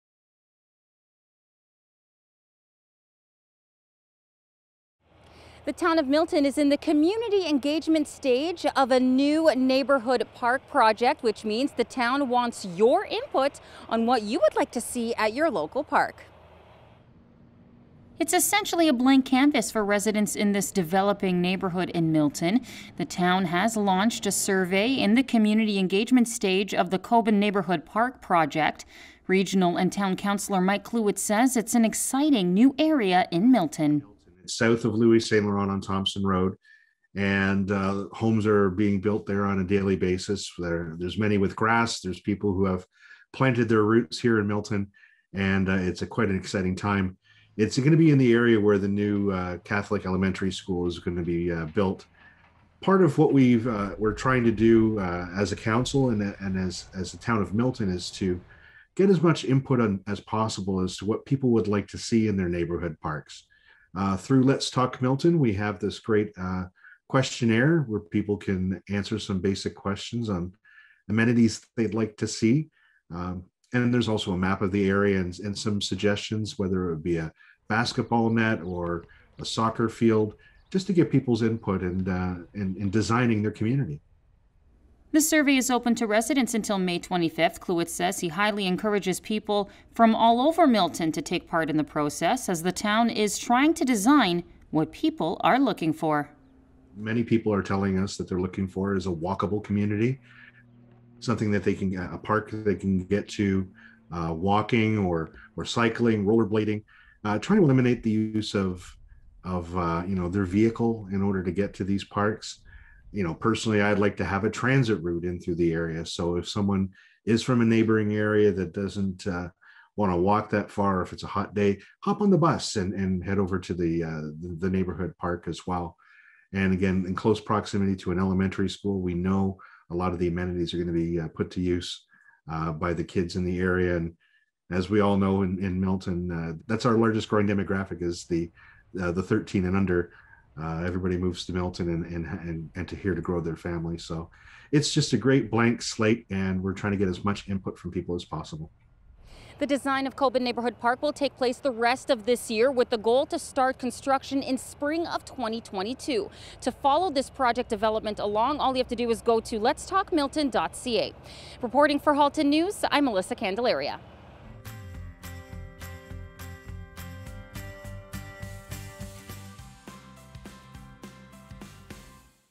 Town of Milton looks to the public for input on new park
The Cobban Neighbourhood Park project is underway and in the community engagement stage of the process. That means the Town of Milton wants to hear your suggestions on what you'd like to see in your park. The community engagement phase runs until May 25. To follow this process and to share your input go to letstalkmilton.ca.